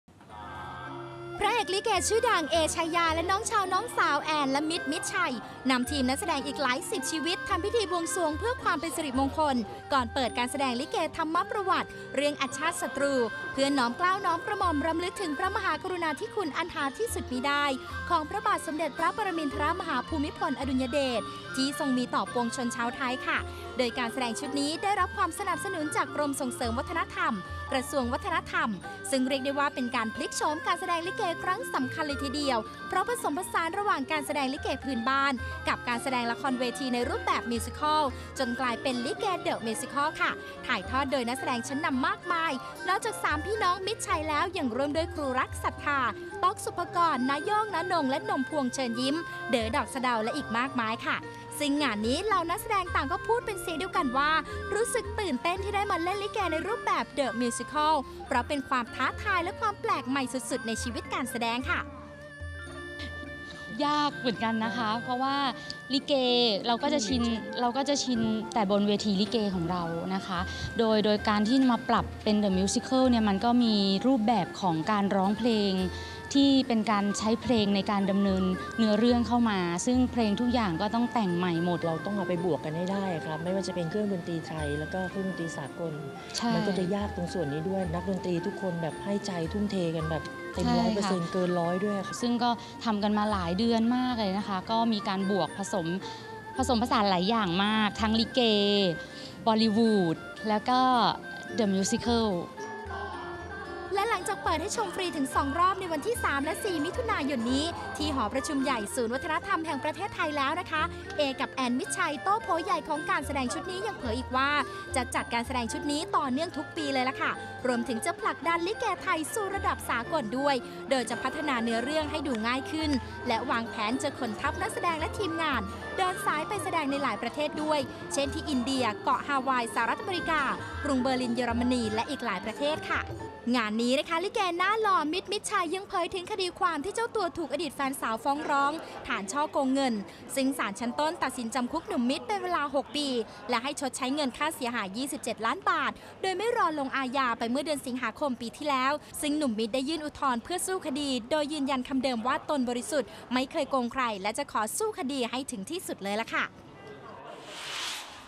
“แอน-เอ” ผุดการแสดงลิเกเดอะมิวสิคัล
พระเอกลิเกชื่อดัง “เอ ไชยา” และน้องชาย น้องสาว แอน และมิตร มิตรชัย ทำพิธีบวงสรวงเพื่อความเป็นสิริมงคลก่อนเปิดการแสดงลิเกธรรมประวัติ เรื่อง “อชาตศัตรู” เพื่อน้อมเกล้าน้อมกระหม่อมรำลึกถึงพระมหากรุณาธิคุณอันหาที่สุดมิได้ของพระบาทสมเด็จพระปรมินทรมหาภูมิพลอดุลยเดช ที่ทรงมีต่อปวงชนชาวไทย โดยการแสดงชุดนี้ได้รับความสนับสนุนจากกรมส่งเสริมวัฒนธรรม กระทรวงวัฒนธรรม ซึ่งเรียกได้ว่าเป็นการพลิกโฉมการแสดงลิเกครั้งสำคัญ เพราะผสมผสานระหว่างการแสดงลิเกพื้นบ้าน กับการแสดงละครเวทีในรูปแบบมิวสิคัล จนกลายเป็นลิเกมิวสิคัล ถ่ายทอดโดยนักแสดงชั้นนำมากมาย นอกจาก 3 พี่น้องมิตรชัยแล้ว ยังร่วมด้วย ครูรัก-ศรัทธา, ต๊อก-ศุภกร, น้าโย่ง-น้านงค์ และนมพวง เชิญยิ้ม / เด๋อ ดอกสะเดา และอีกมากมาย